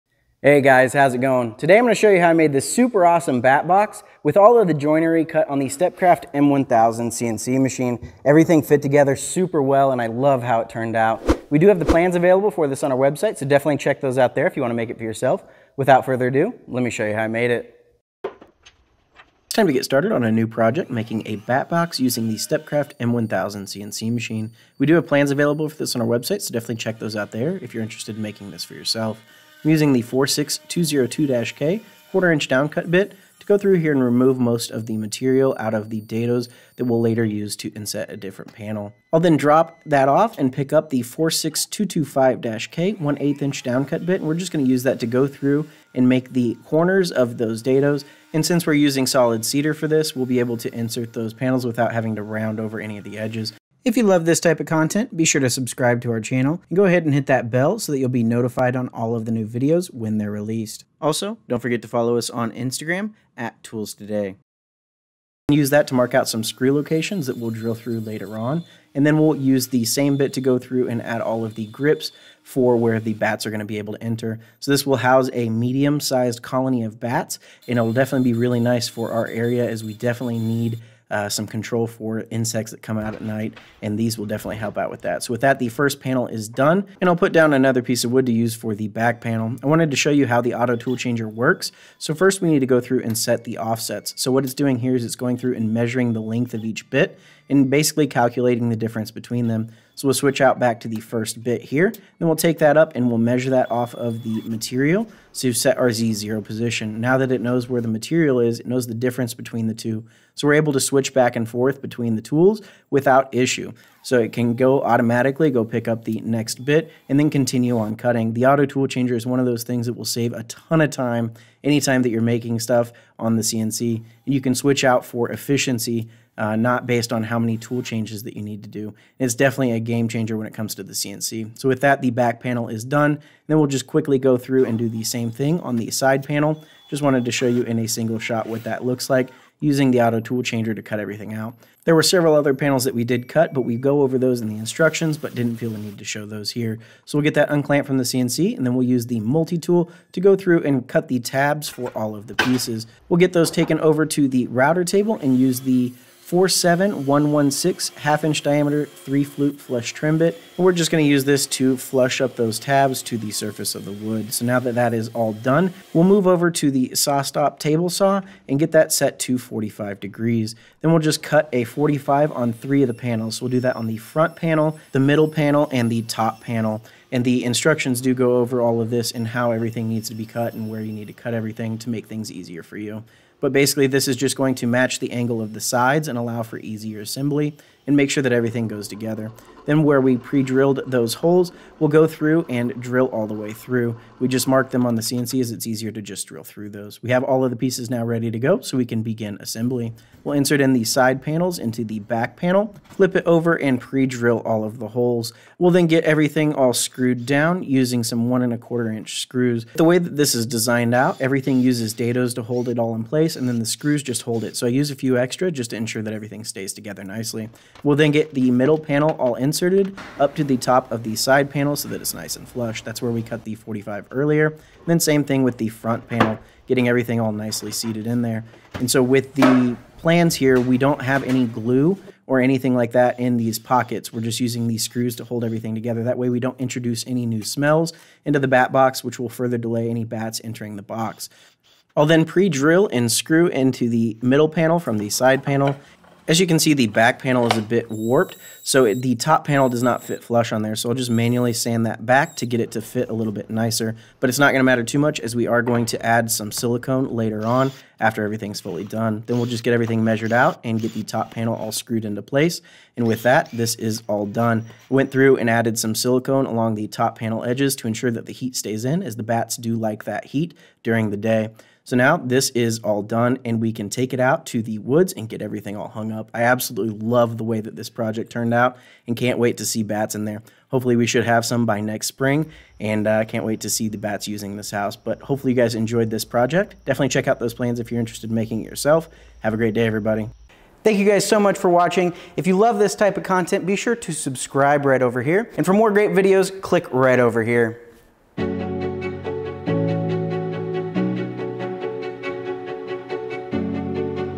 How to Make a Bat Box | ToolsToday
We have a large property and one of the best natural ways to keep mosquitos out is their predator, the bat! Bat’s need a place to call home, so today I’m making a Bat Box with Amana Tool® CNC router bits and saw blades on my STEPCRAFT M.1000 CNC machine.

The plans for this project are also available on our website item no. BAT_BOX

Feed, Speed & CNC Running Parameters

Amana Tool 46202-K SC Spektra Extreme Tool Life Coated Spiral Plunge 1/4 Dia x 3/4 CH x 1/4 SHK 2-1/2 Inch Long Down-Cut Router Bit
RPM: 18,000
Feed Rate: 100ipm
Plunge Rate: 50ipm
Spiral Ramp Down: 0.250”

Amana Tool 46225-K SC Spektra Extreme Tool Life Coated Spiral Plunge 1/8 Dia x 13/16 CH x 1/4 SHK 2-1/2 Inch Long Down-Cut Router Bit
RPM: 18,000
Feed Rate: 60ipm
Plunge Rate: 30ipm
Depth per pass: 0.125”

Amana Tool 47116 Carbide Tipped Flush Trim 1/2 D x 1/2 CH x 1/4 Inch SHK w/ Lower Ball Bearing 3-Flute Router Bit

Cutting a 45° angle on a piece for the Bat Box using the sliding cross cut table attachment, item no. TSA-SA48, on the SawStop table saw with the Amana Tool no. 610801C 10" diameter 80T ElectroBlu coated crosscutting blade